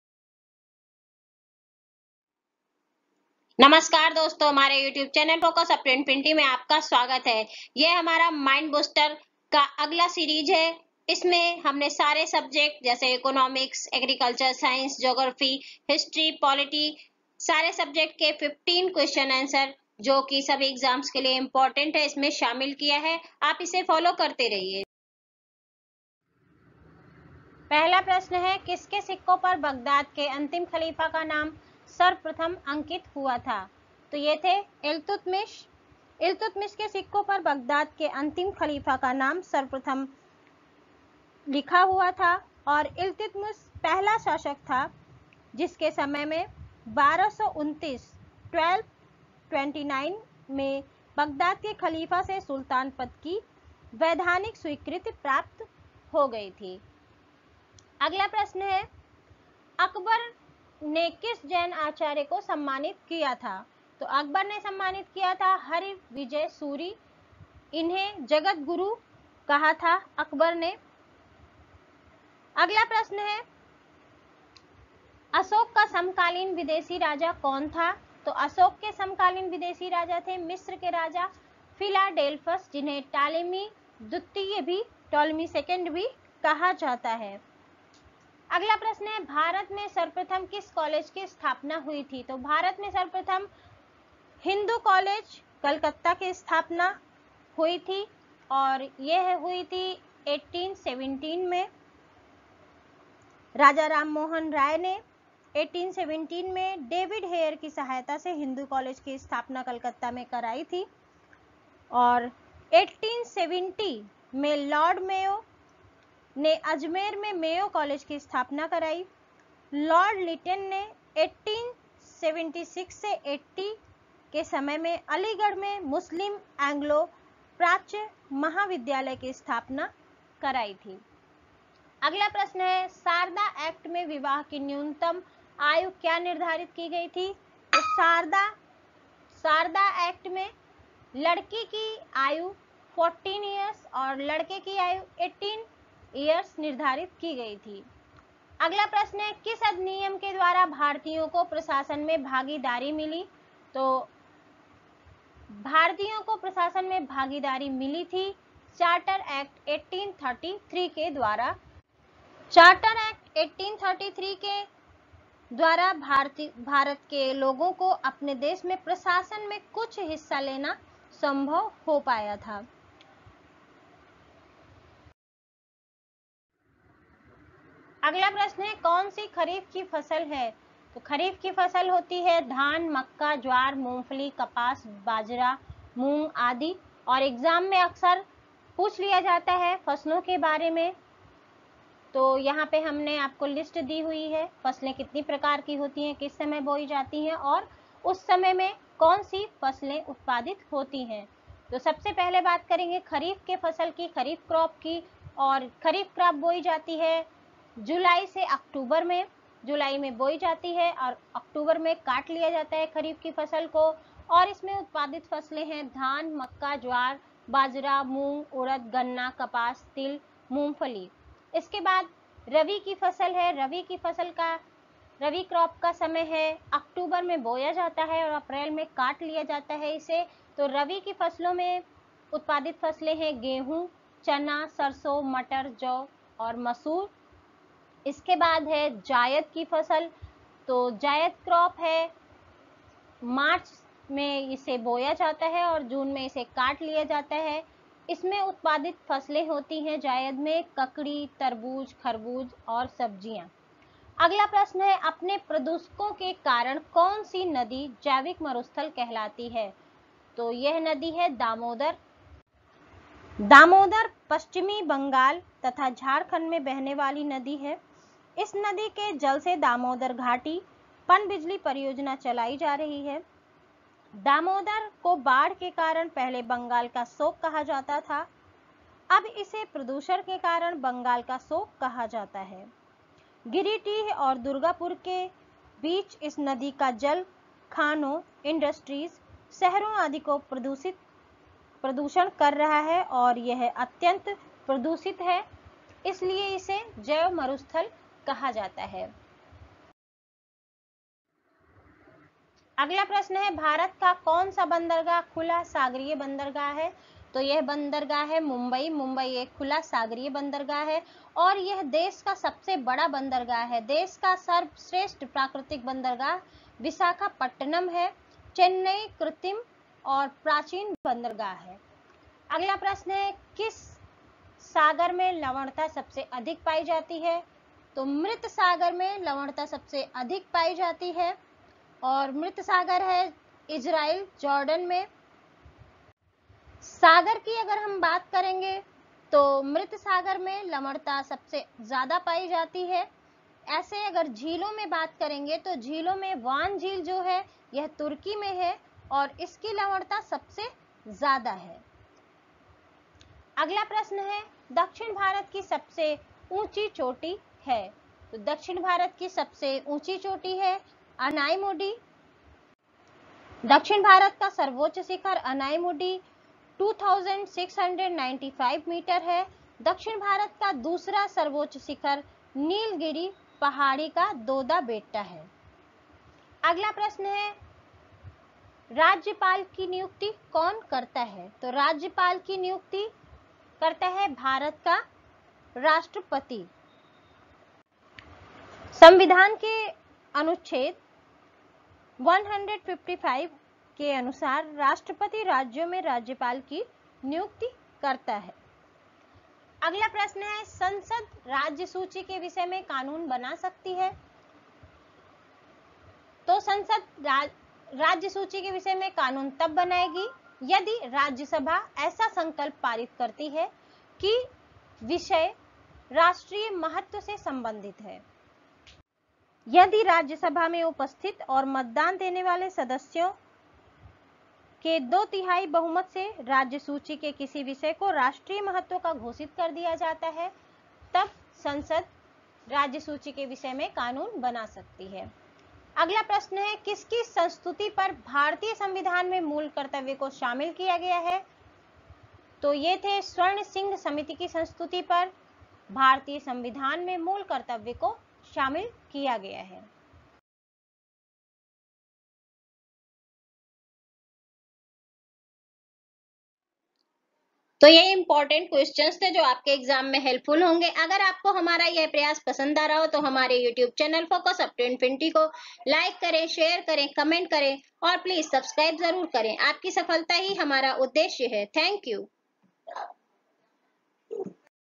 GS/24 : MIND BOOSTER DAILY GENERAL STUDIES/GENERAL KNOWLEDGE FOR ALL COMPETITIVE EXAMS
IN THIS I WILL PROVIDE YOU WITH DAILY GENERAL STUDIES/GK QUESTION/ANSWER PART 24 KNOWN BY NAME MIND BOOSTER WHICH WILL BE USEFUL FOR ALL TYPE OF COMPETITIVE EXAMINATIONS LIKE UPSC IAS, STATE PCS, SCC, RAILWAY, BANK EXAMS, TEACHING RELATED EXAMS AND ALL OTHER EXAMS.